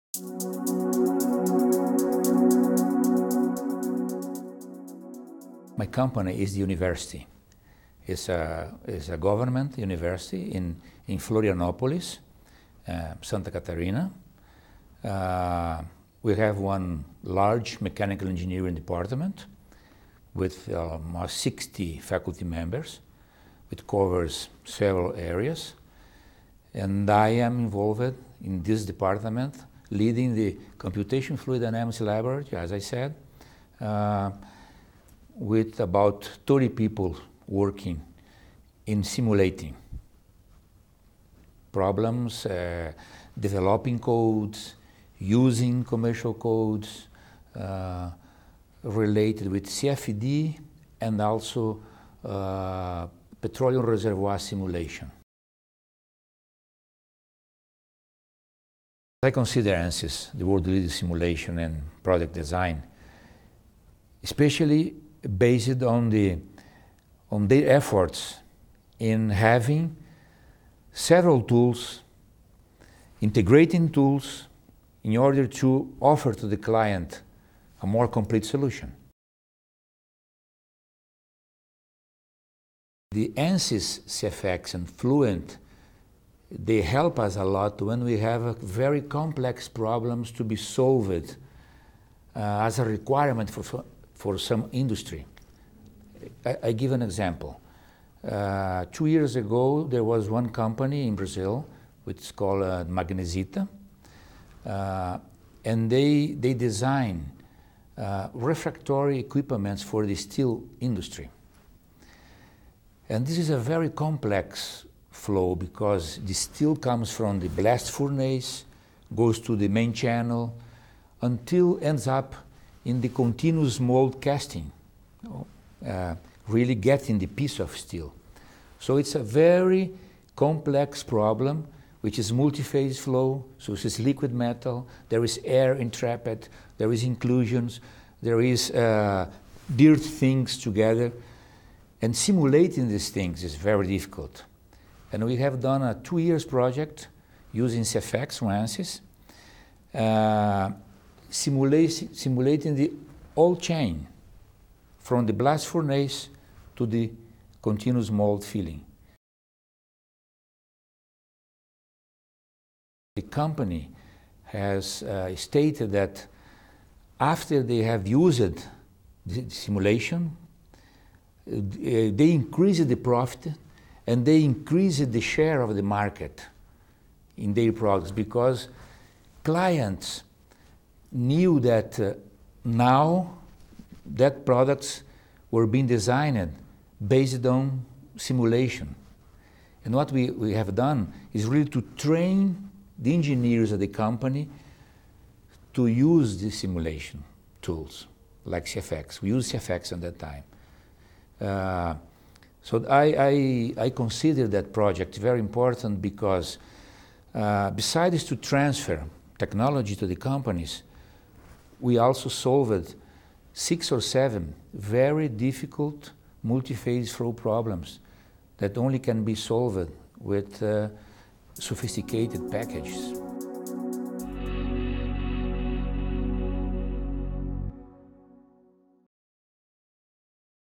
Federal University of Santa Catarina and ANSYS [Case Study]
The Federal University of Catarina in Brazil has a large engineering department that uses simulation from ANSYS. The software assists the university by solving very complex industrial problems for companies that increase profits and market share for those organizations.